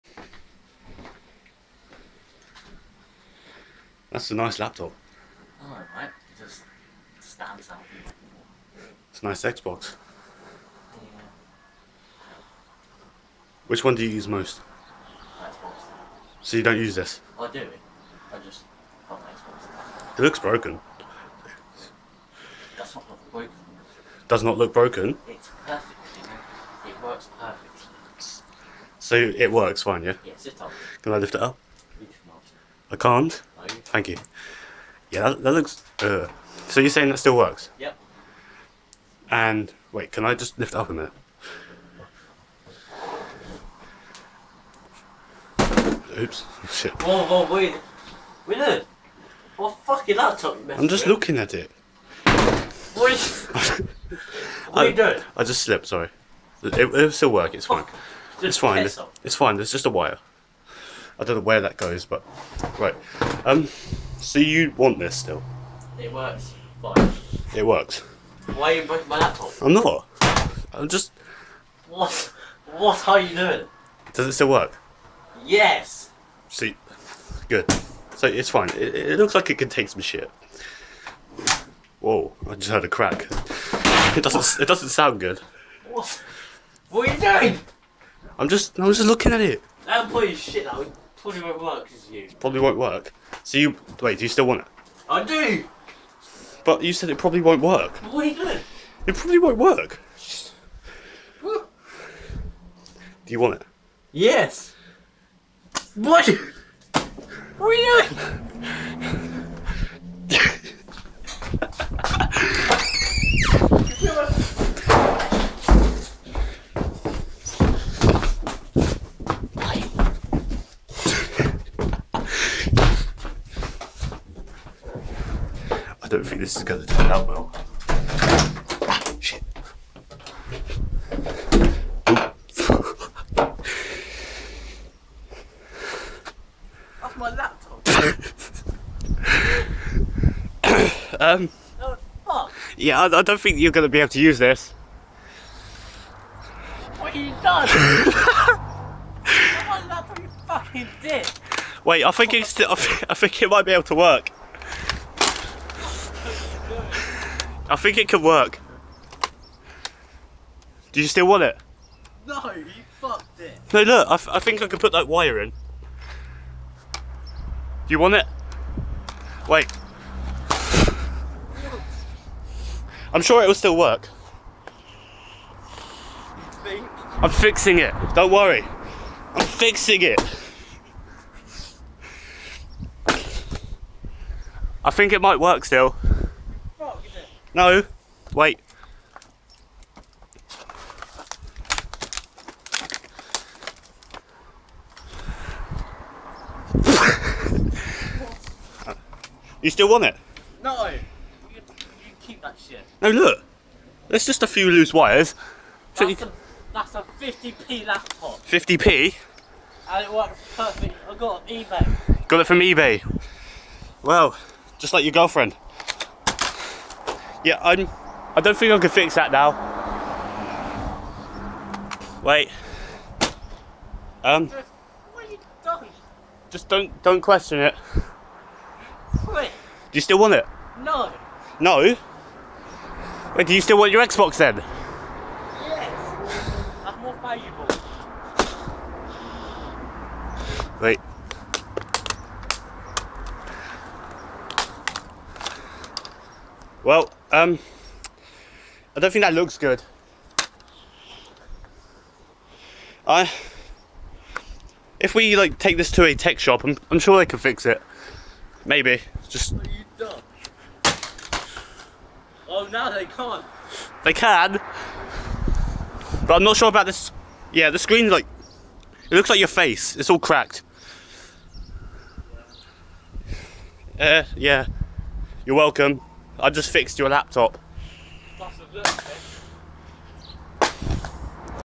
THE LAPTOP IS DEAD (Funny Video!)
I don't think the laptop works anymore, this was genuinely his laptop and it did work before I did all that, but don't worry, he still has an xbox. The laptop was worth £80 as it was about a year old haha, sorry if anyone takes offense to this, bit it was just for fun, well kind of haha, sorry to my brothers laptop, but I do not care.